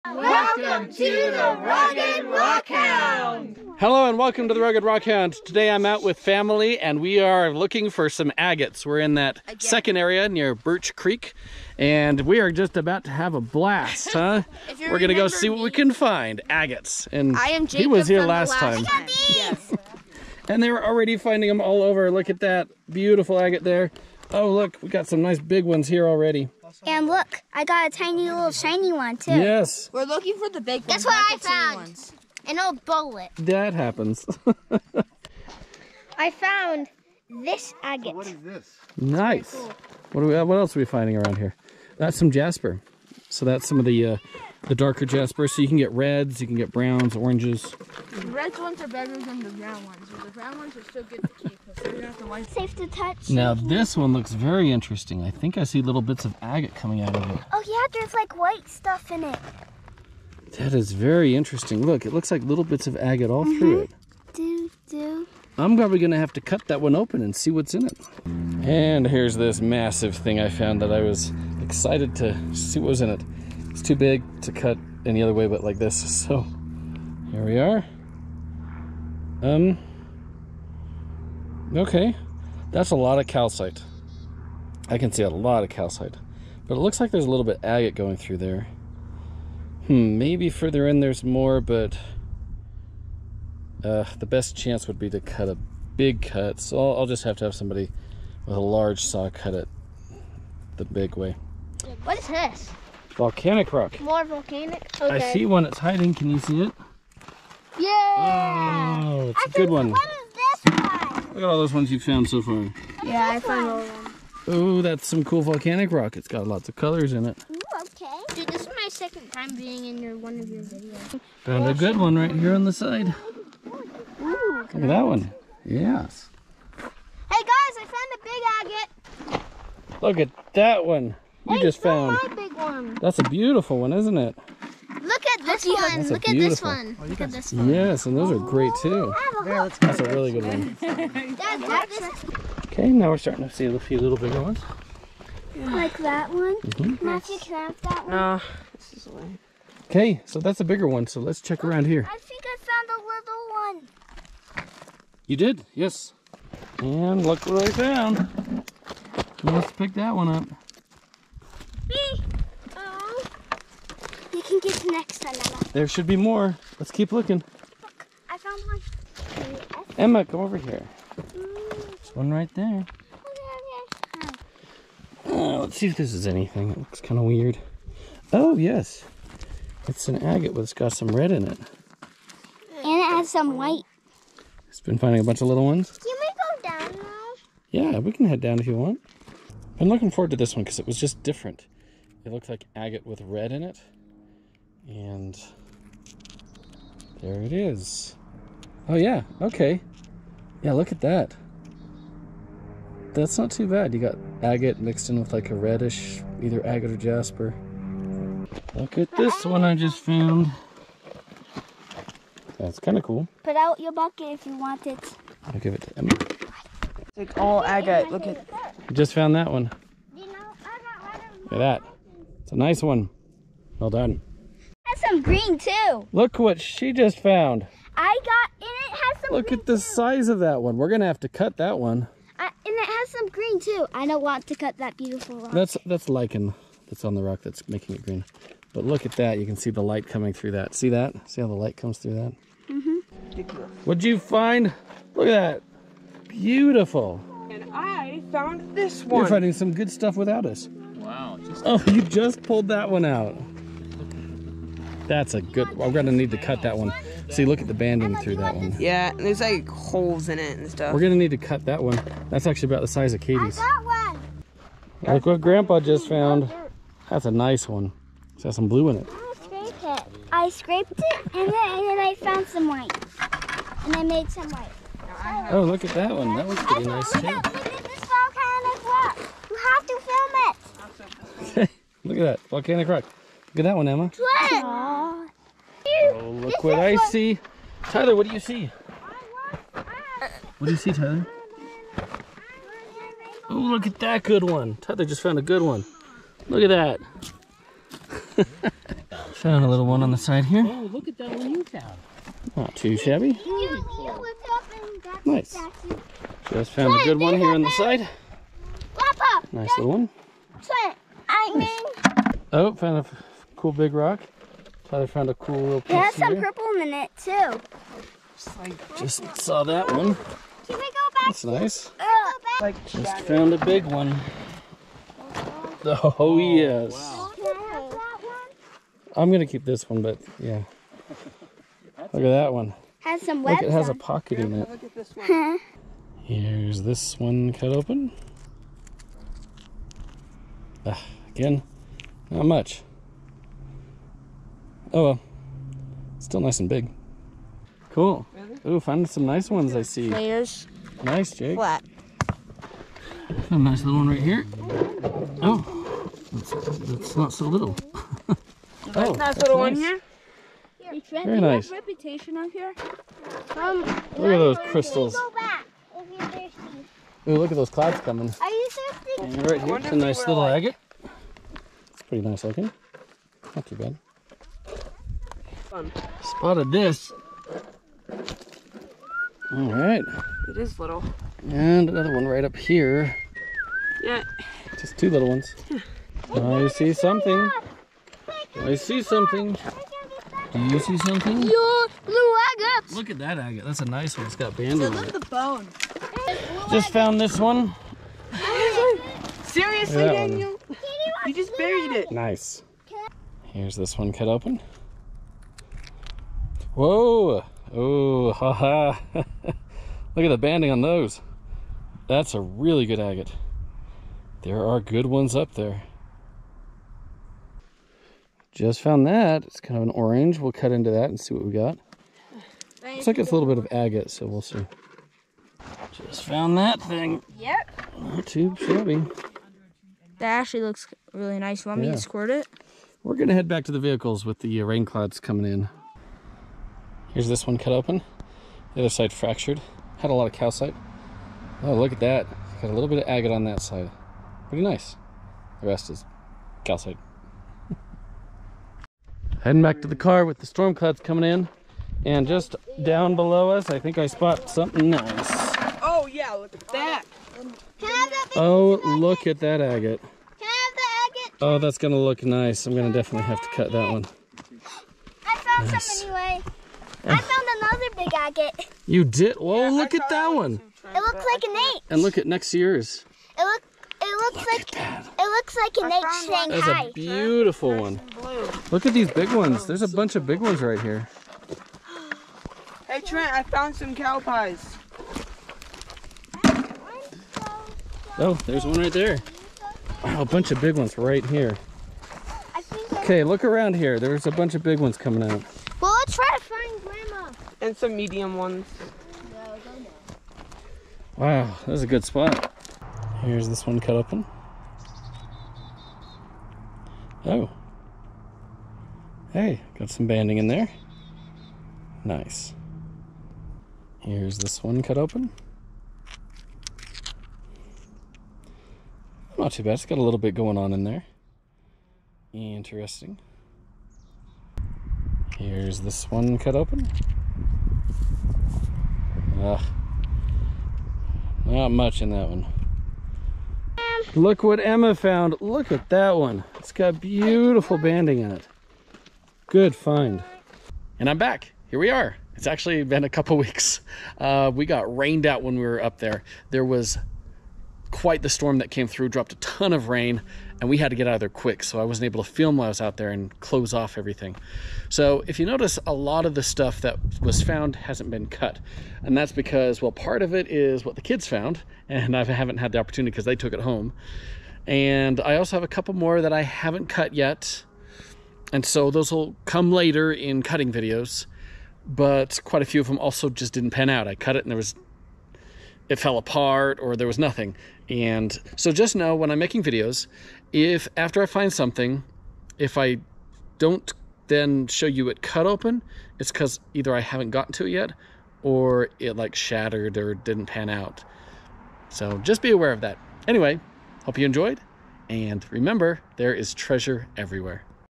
Rockhounding for Agates at Birch Creek With Friends and Family!
GPS coordinates of where we parked:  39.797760, -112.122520